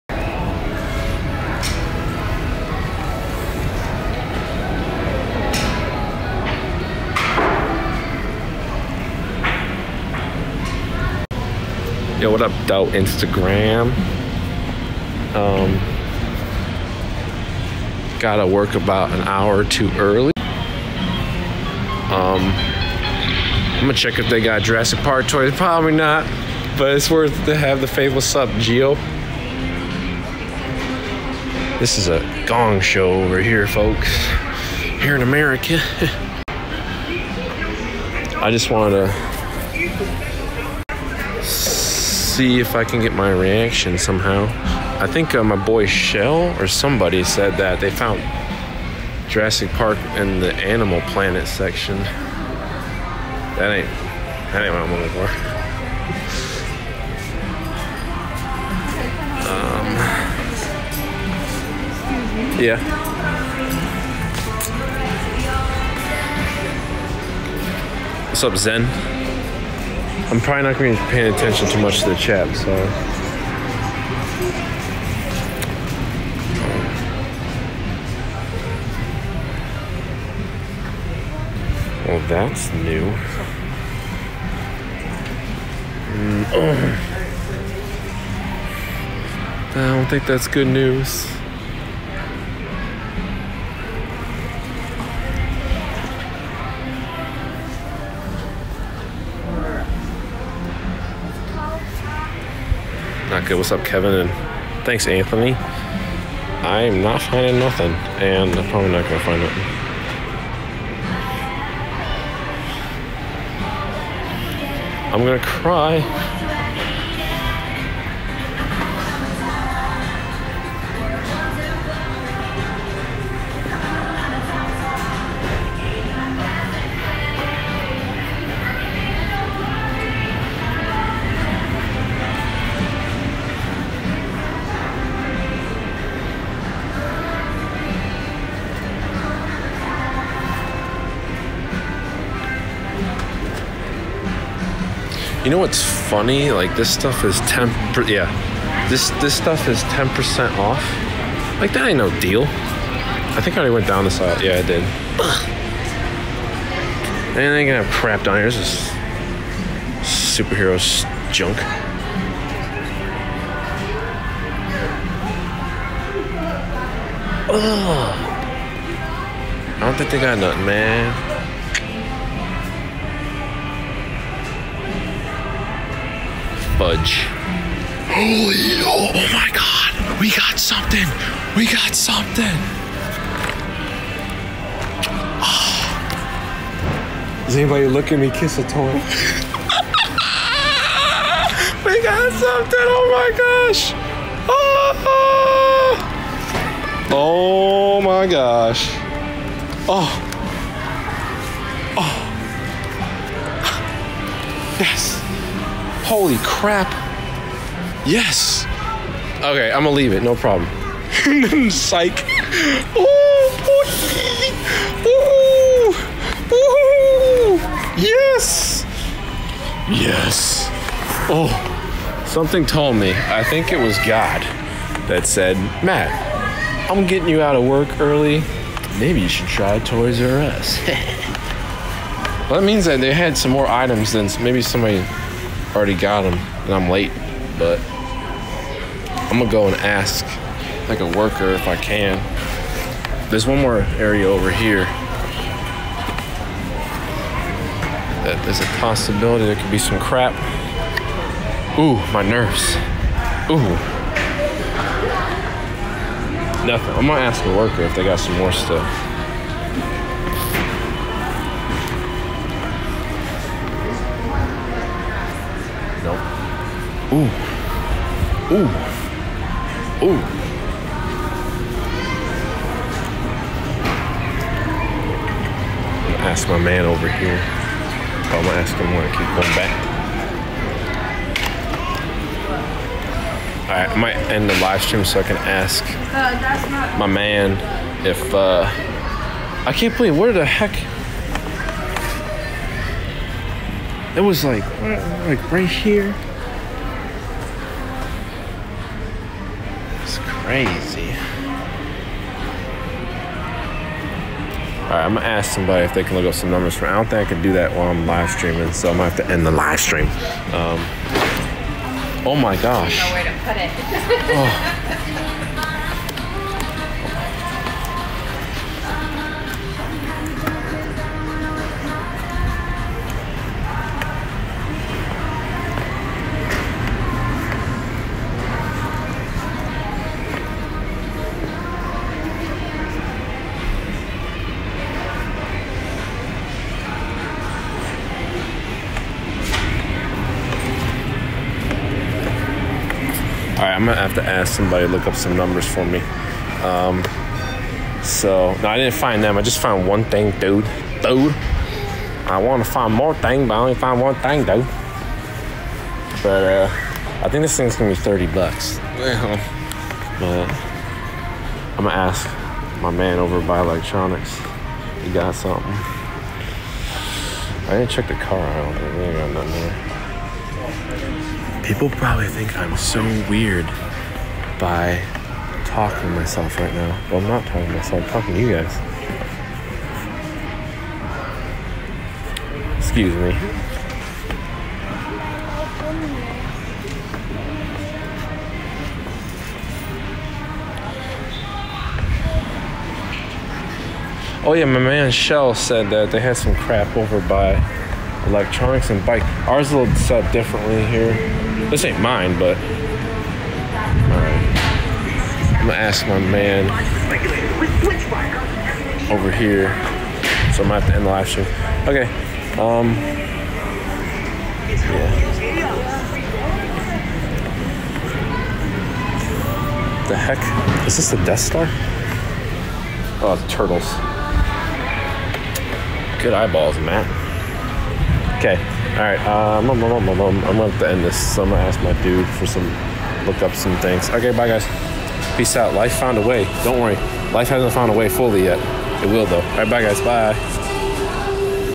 Toys "Were" Us - The Kingdom Has Fallen
An archived Instagram Livestream of toy hunting for Mattel's Jurassic World: Fallen Kingdom toys, on 04/02/18 from Toys'R'Us.